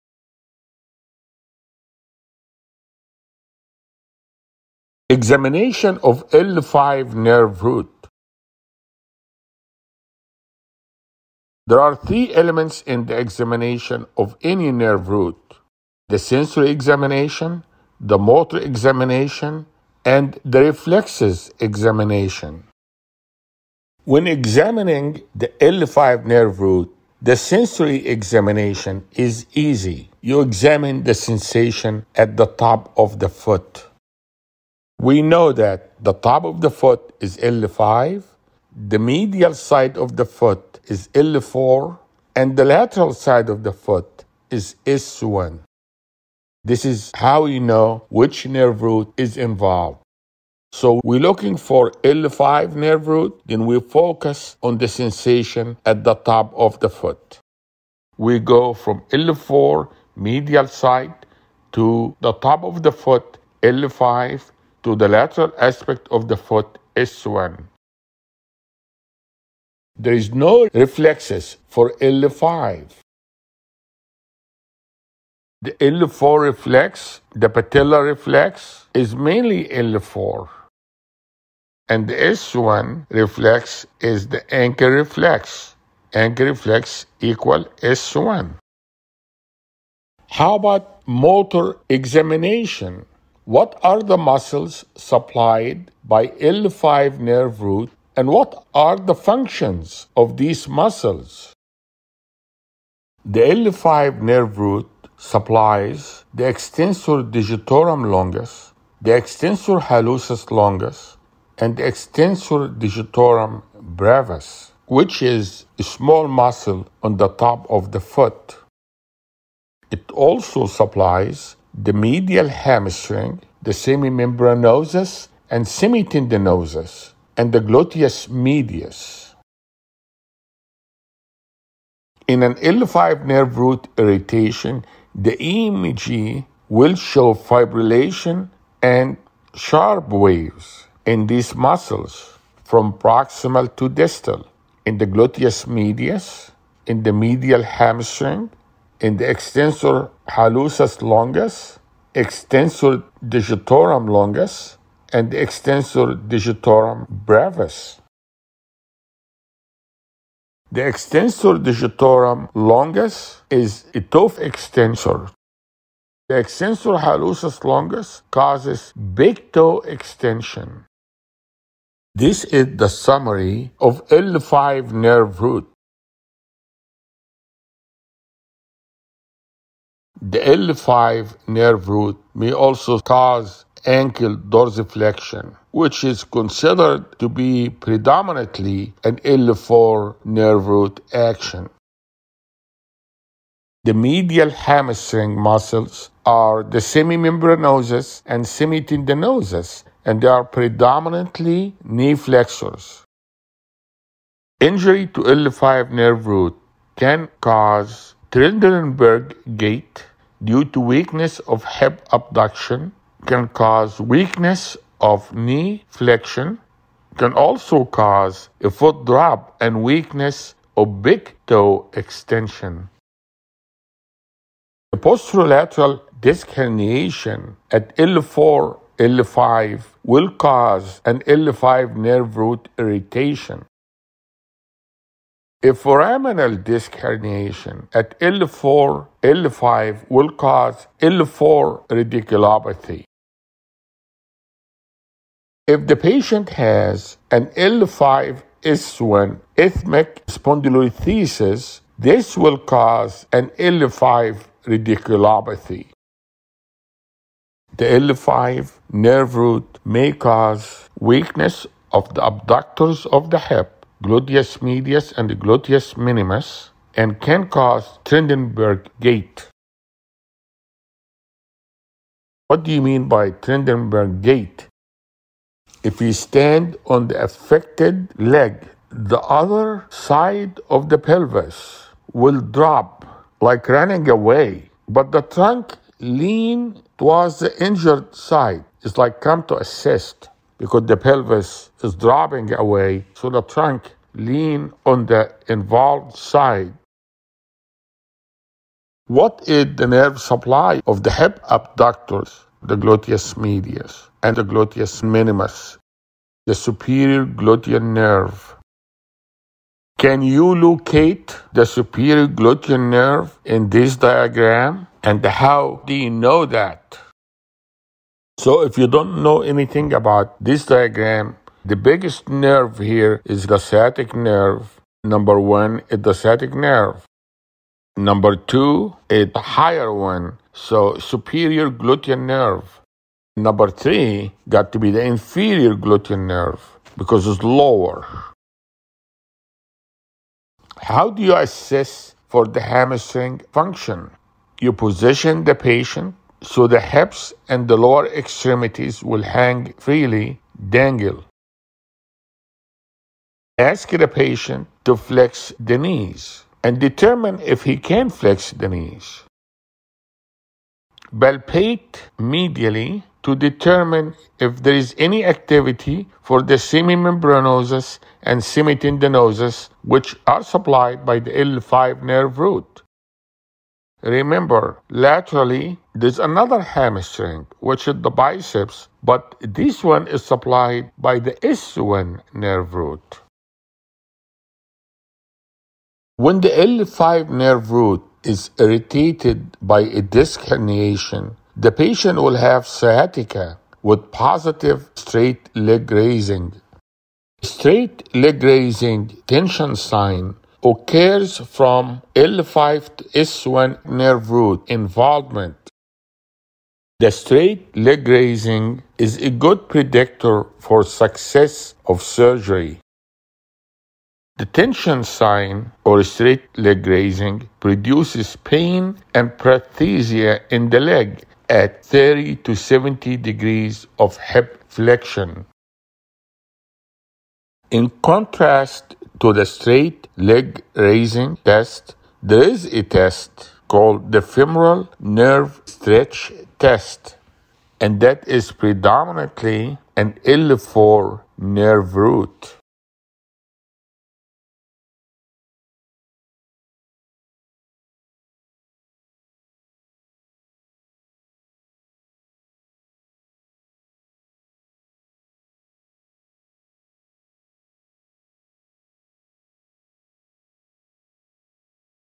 Examination Of L5 Nerve Root - Everything You Need To Know - Dr. Nabil Ebraheim
Dr. Ebraheim’s educational animated video describes examination of the L5 nerve root.